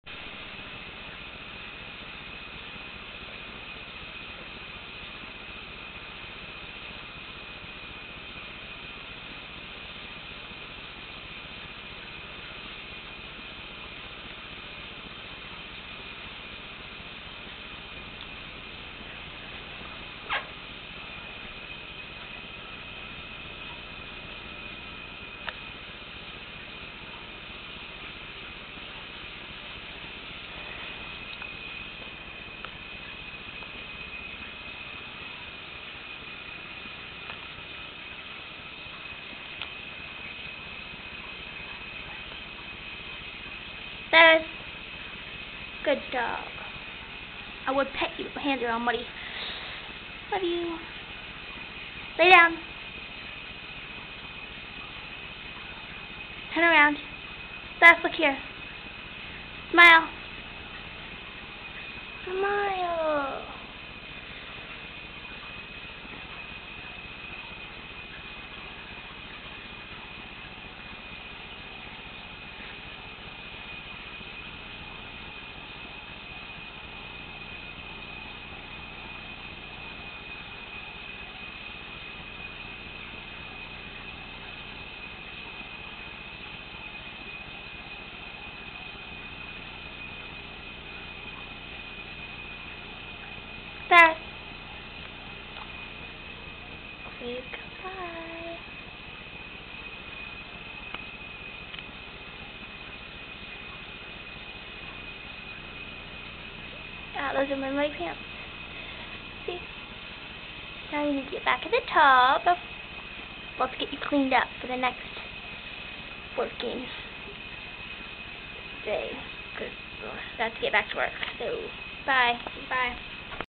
Cyrus Dog!
This is  Cyrus in a bathroom floor